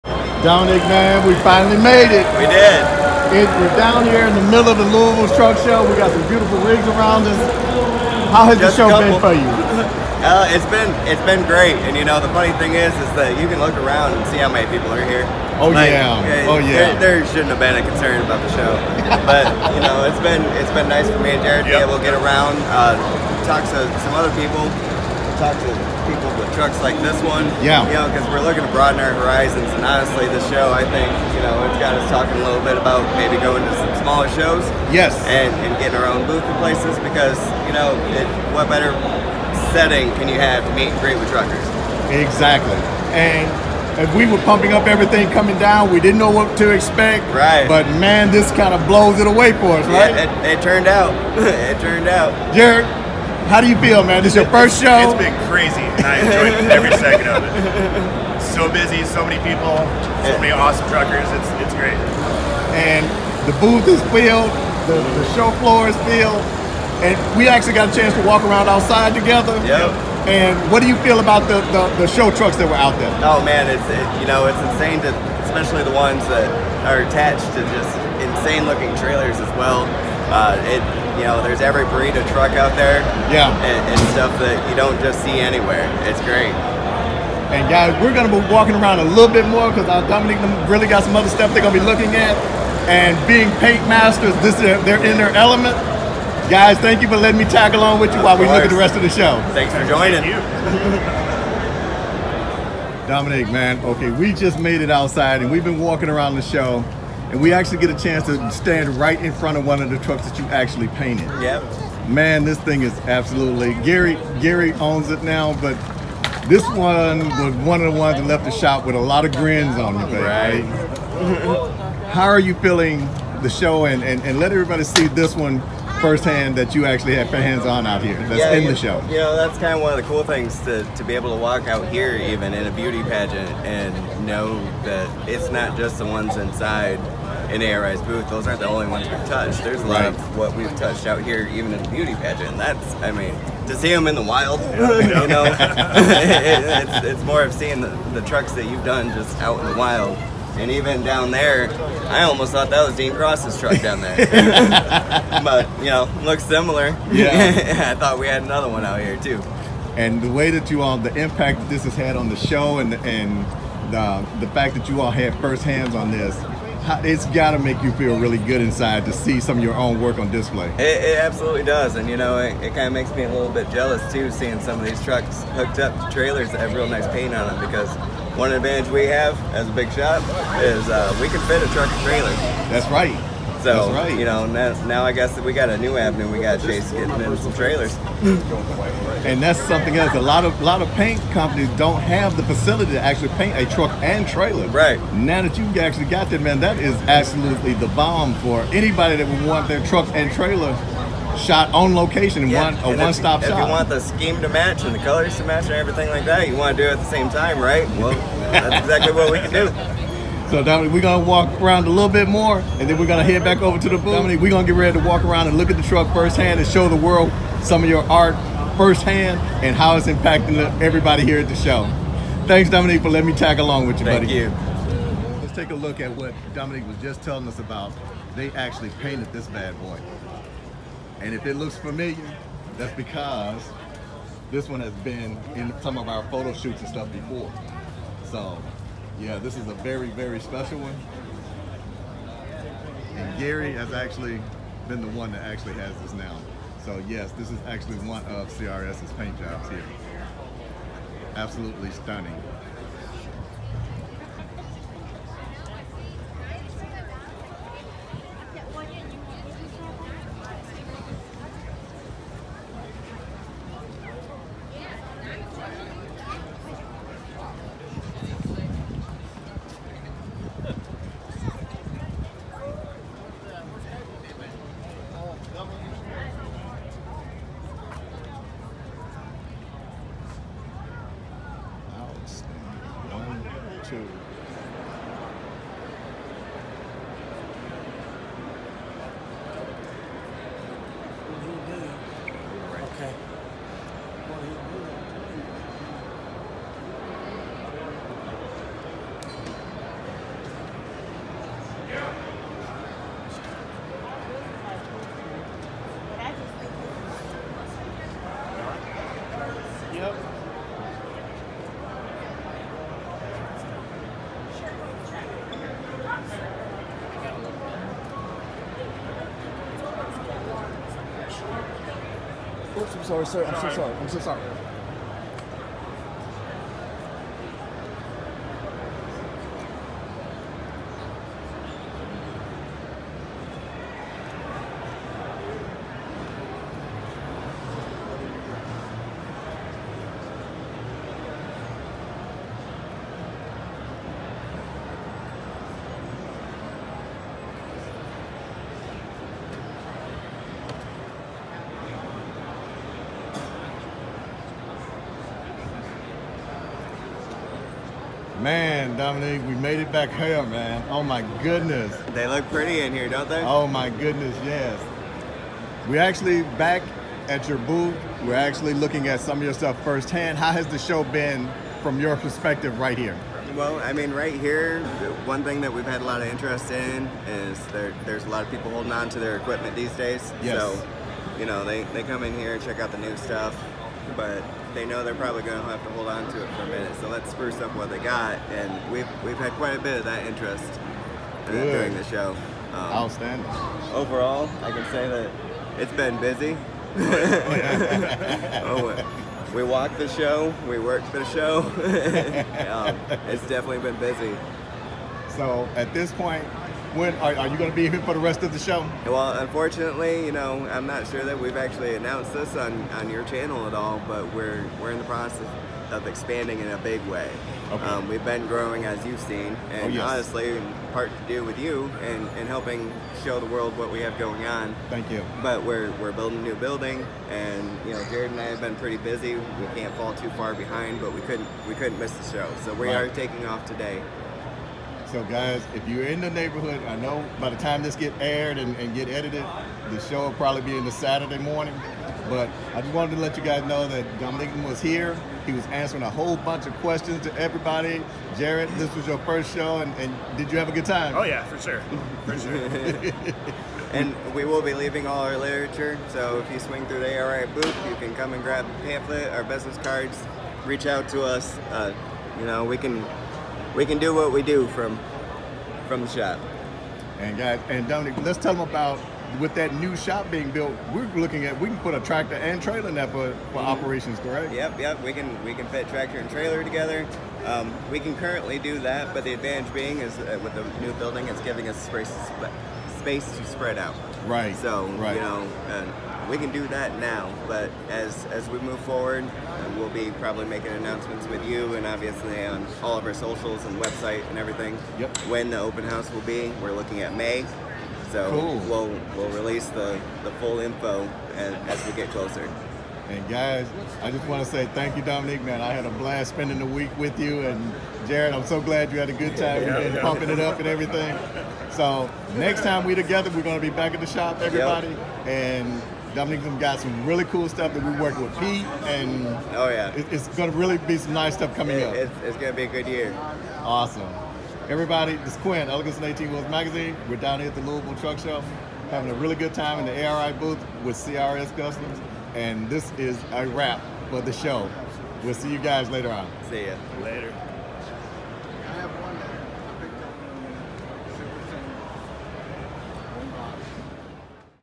Hanging out with the CRS crew at MATS 2024
Had the opportunity to walk the show with team CRS , what an awesome time. Welcome aboard Jarad